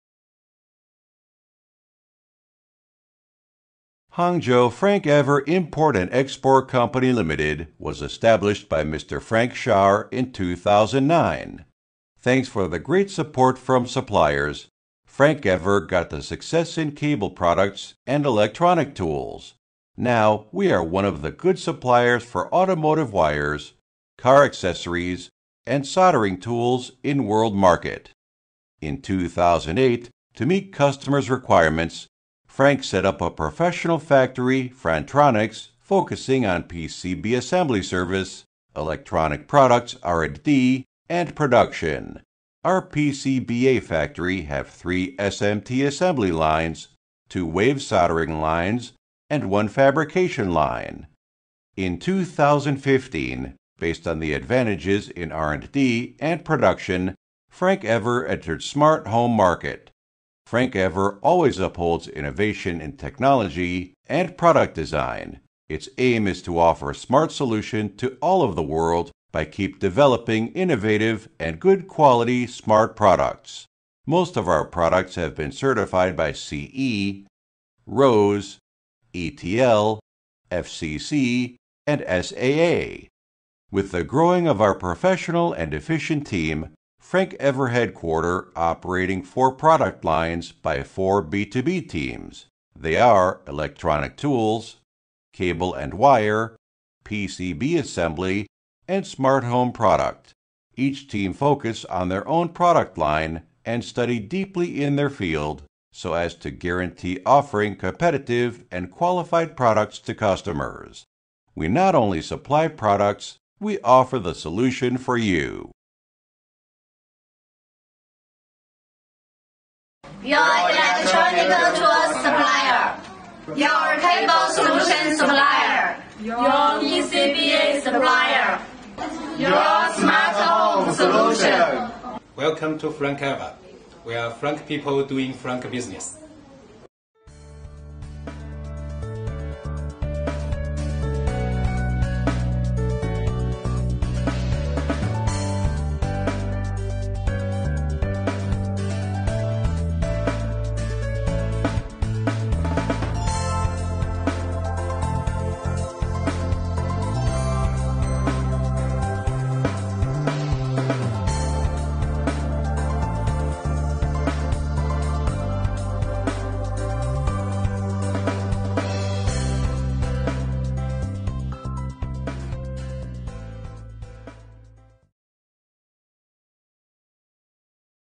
FRANKEVER company video
Hangzhou Frankever Imp. & Exp. Co., Ltd. was established by Mr. Frank Shar in May, 2009.

Thanks for the great support from suppliers, Frankever got the success in cable products and electronic tools. Now we are one of the top suppliers for electronic tools in world market.

In 2008, to meet customers’ requirements, Frank set up a professional factory Frantronix focusing on P C B assembly service, electronic products R & D and production. Our P C B A factory have 3 SMT assembly lines, 2 wave soldering lines and 1 fabrication line.

With the growing of our professional and efficient team, now Frankever headquarter is operating 4 product lines by 4 B 2 B teams, they are electronic tools, cable and wire, P C B assembly, and smart home product. Each team focus on their own product line and study deeply in their field always so as to guarantee offering competitive and qualified products to customers. We not only supply products, we offer the solution for you.